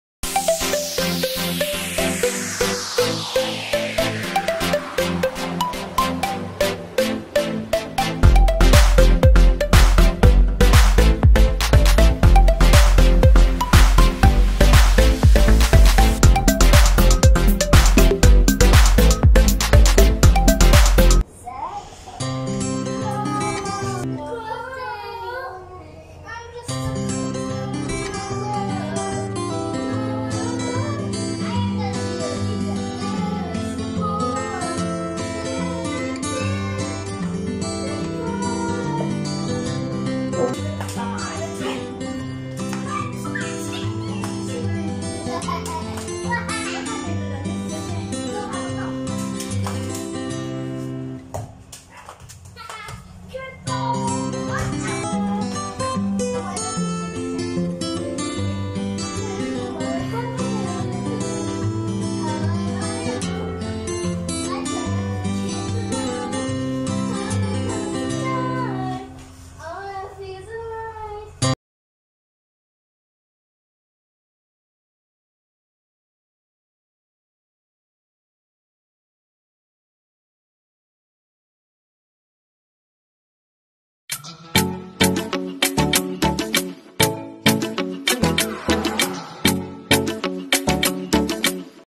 Enjoy The Bonding Time with them/Back to Childhood Playtime Memories
Its great to play back those memories childhood time, fun and enjoy with this kids.